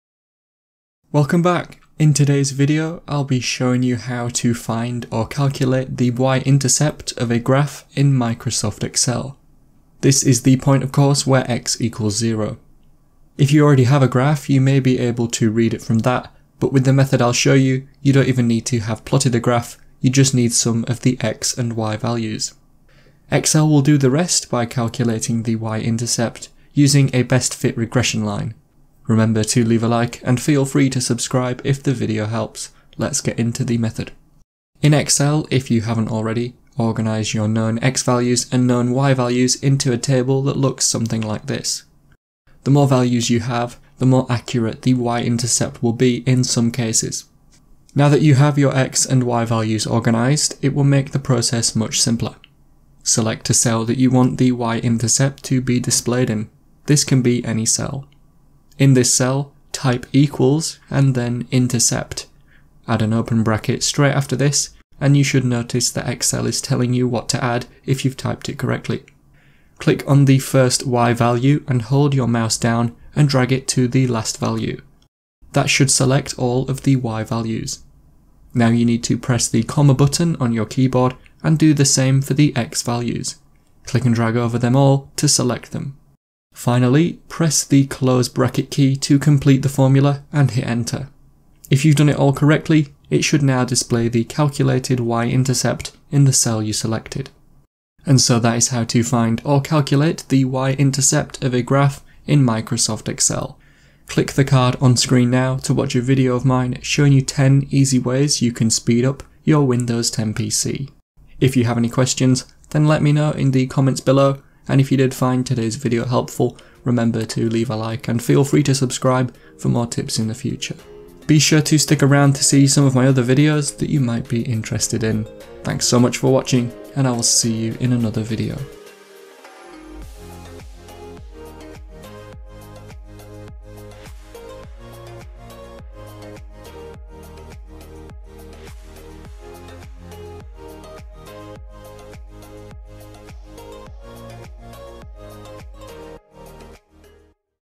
How to Find/Calculate Y Intercept on a Graph in Microsoft Excel
Want to know how to calculate/find the Y intercept of a graph in Microsoft Excel? This video will show you how to get the Y intercept of a line/graph on Excel, using the INTERCEPT formula. This Excel formula will find the Y intercept of a graph using just the known X and Y values. This means you can use this to find the Y intercept with two points.